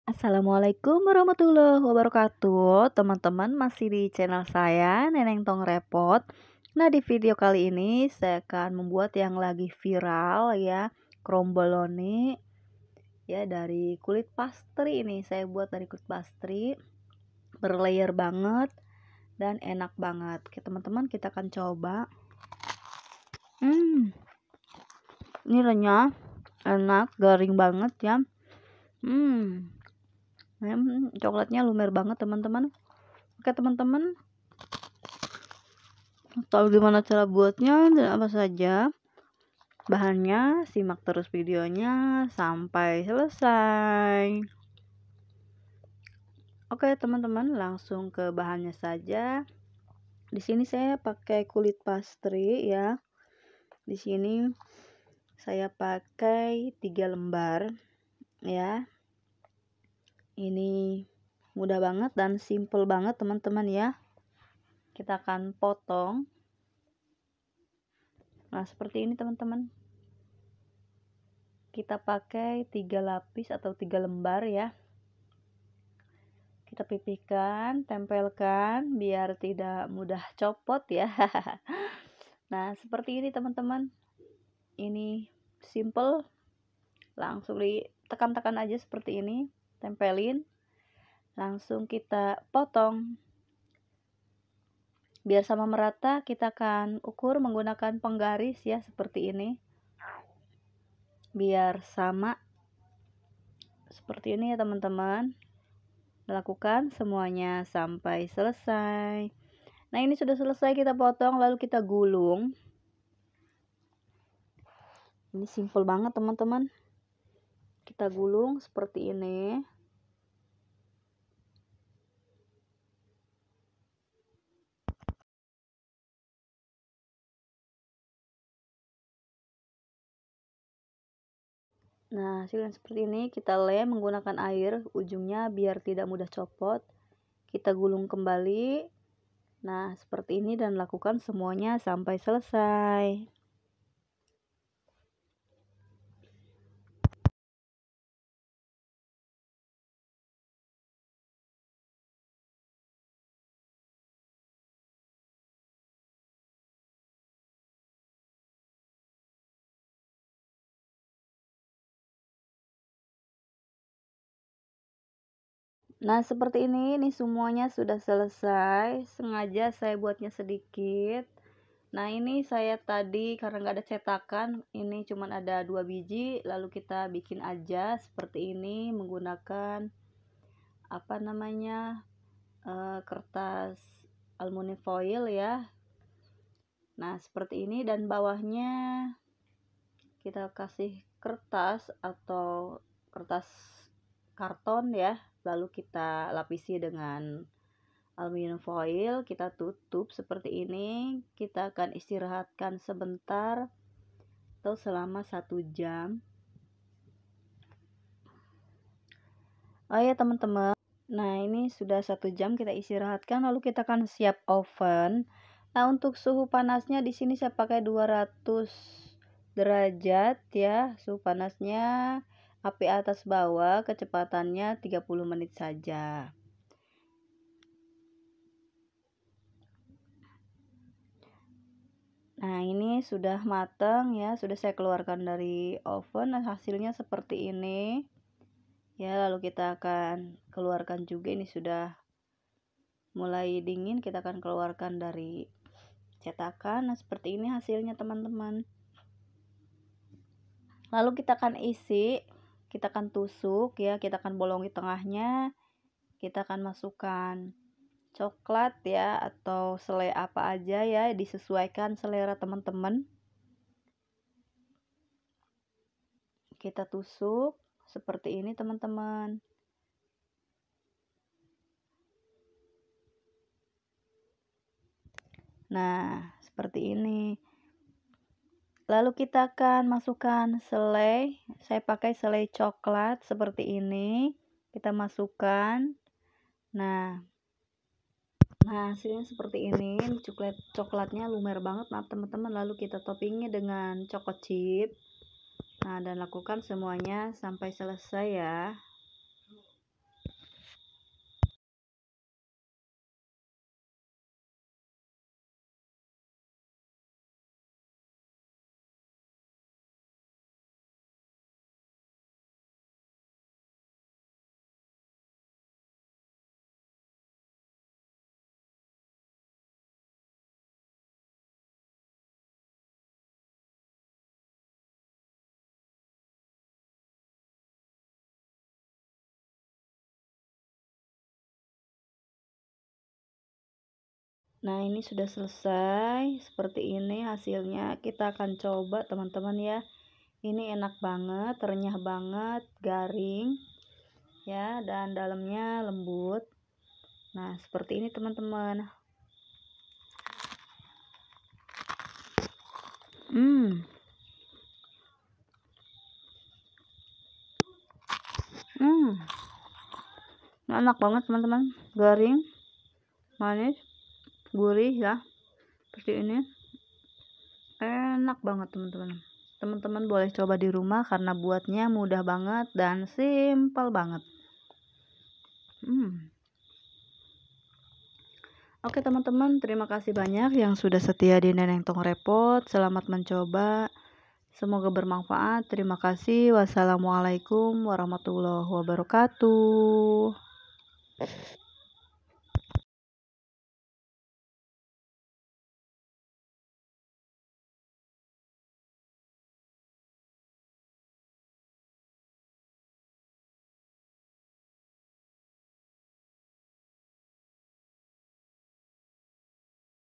1 BAHAN AJA‼️CROMBOLONI VIRAL
BAHAN
3 lembar Kulit pastry shéets
coklat lumer